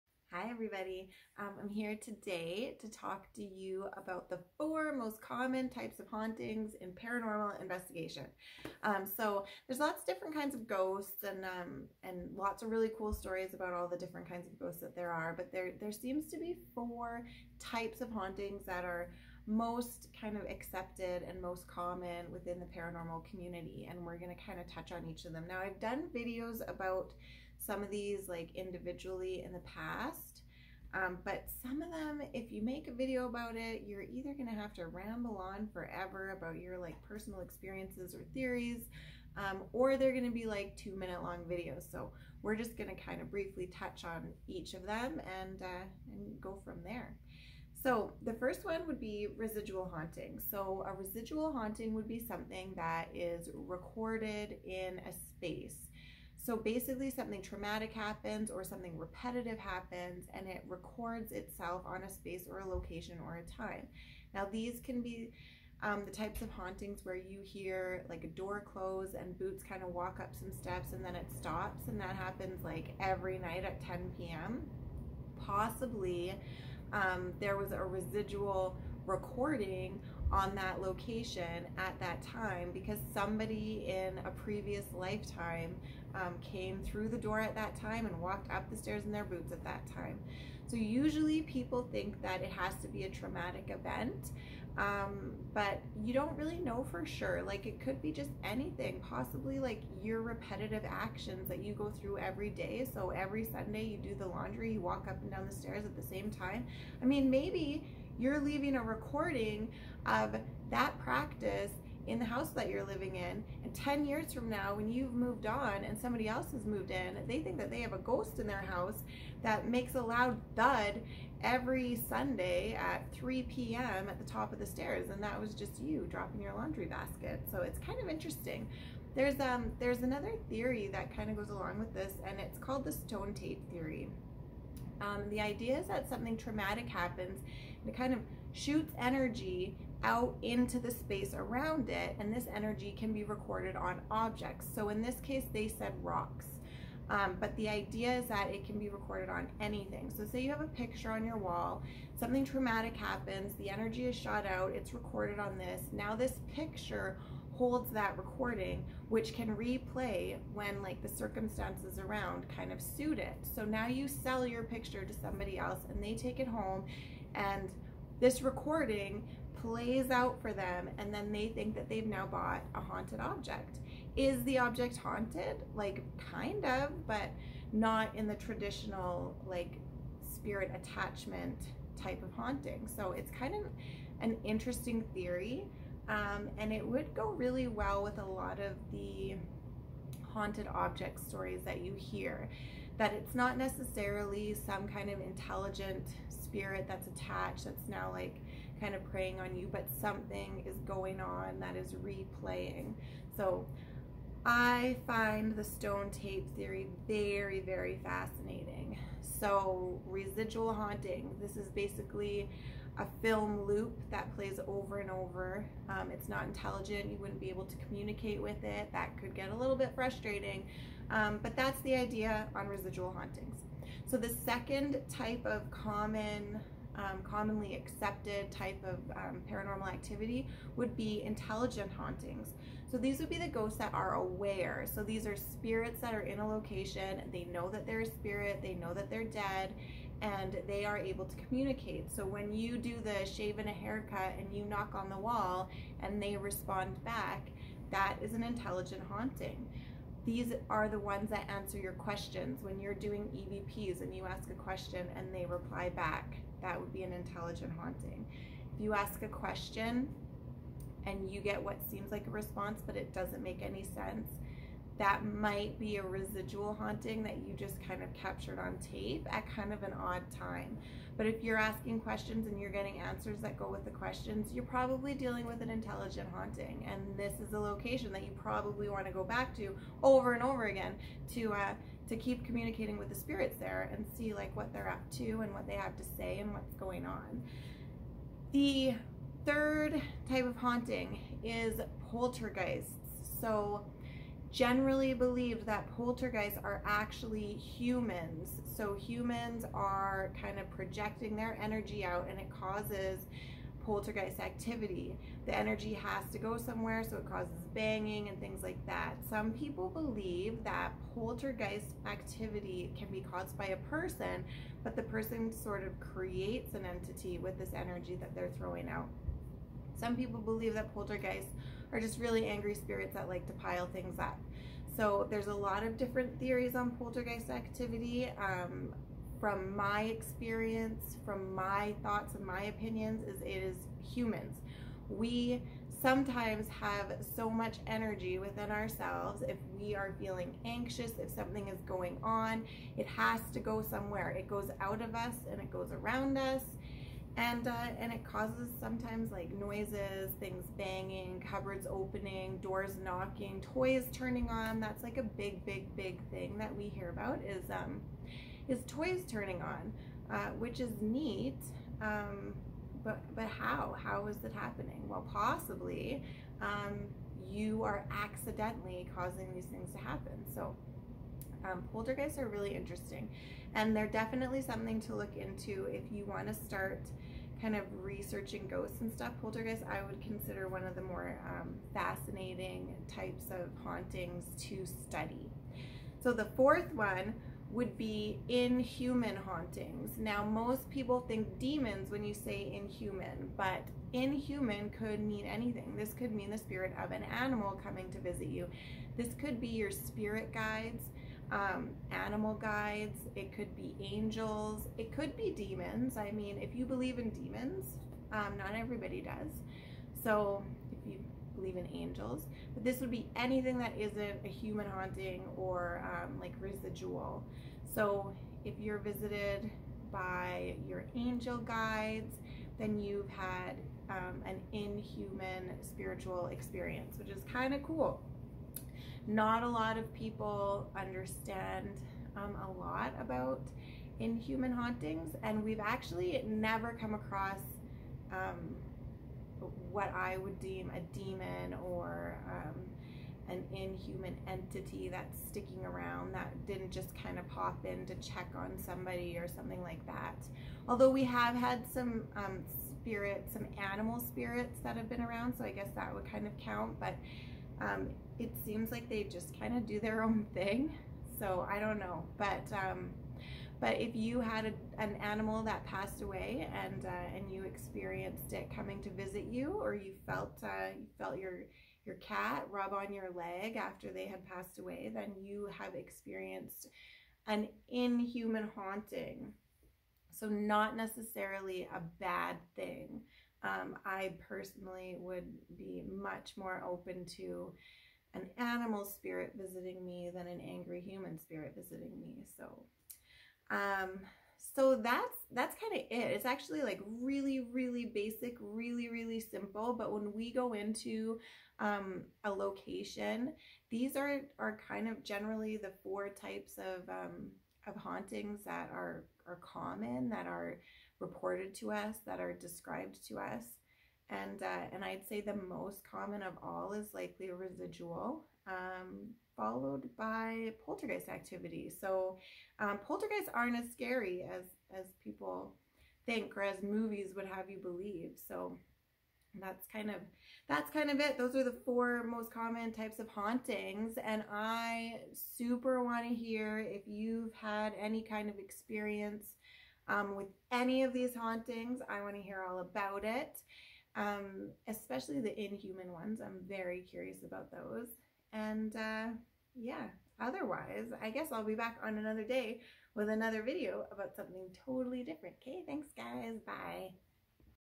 The Four Most Common Spiritual Experiences
Today I touch briefly on the four most common spiritual experiences.
I'd love to hear your paranormal stories!